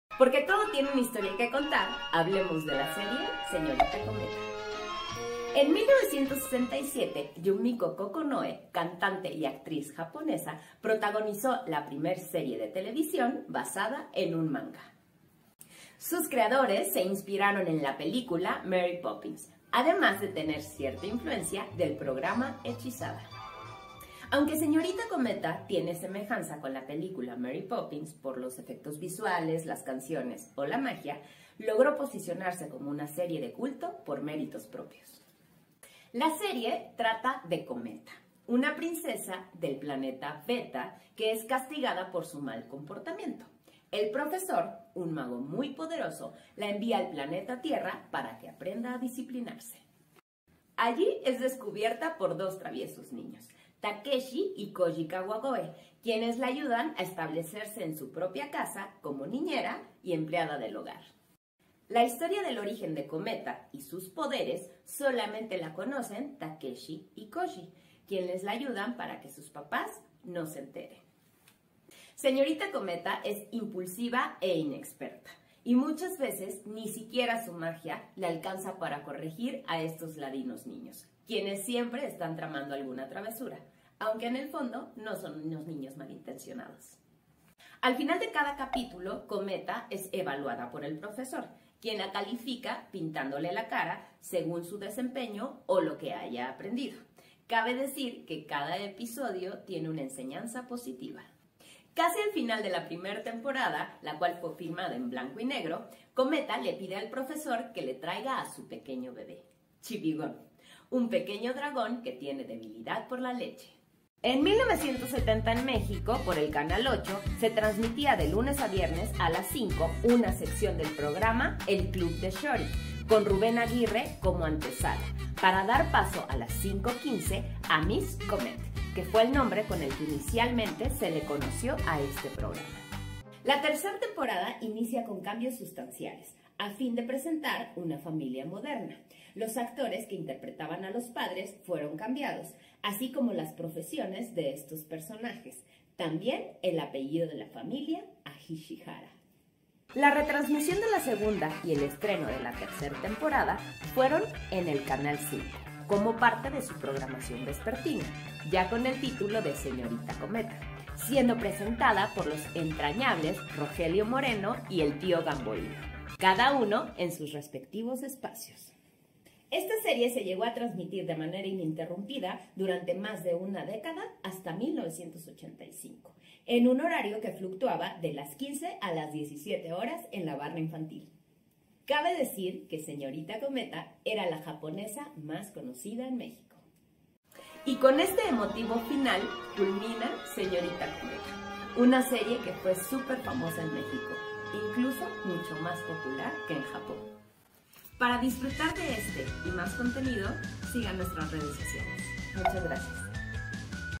Porque todo tiene una Historia que contar: SEÑORITA COMETA.
La serie Señorita Cometa marcó a toda una generación de telespectadores juveniles. ¿Viste ésta serie?
Conductora: Verónica De Alba.